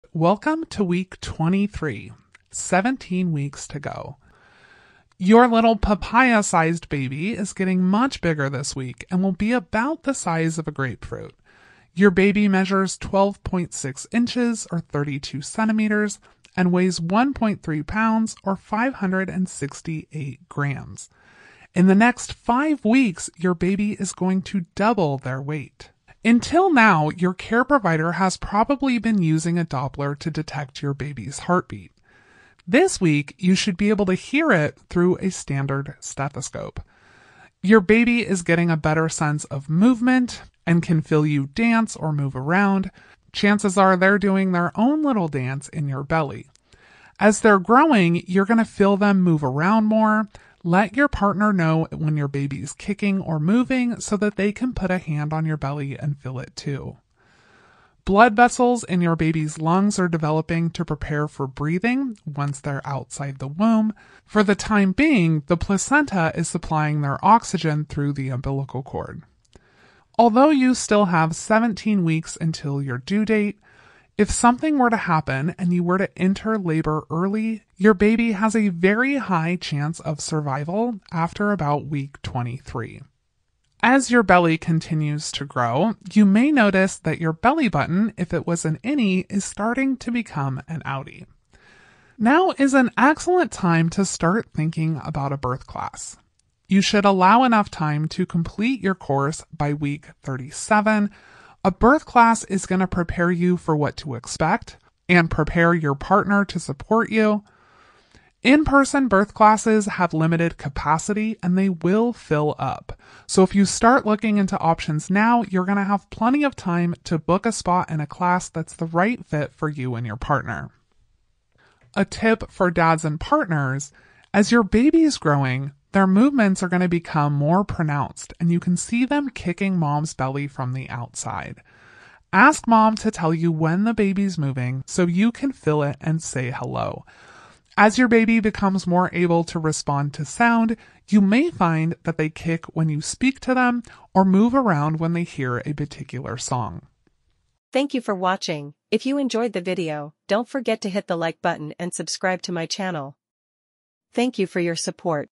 Pregnancy! Week 23 - You should sing songs -
Welcome to Week 23 of pregnancy! In this update, we explore exciting developments for both baby and mom. Learn how your baby is growing to the size of a grapefruit, with increased movement and even the ability to hear their heartbeat through a stethoscope. Discover the marvels of modern medicine ensuring high survival rates for premature births after week 23. For moms, we discuss potential changes in belly button appearance and the importance of considering birth classes. Dads and partners, get ready to feel those kicks and bond with your baby!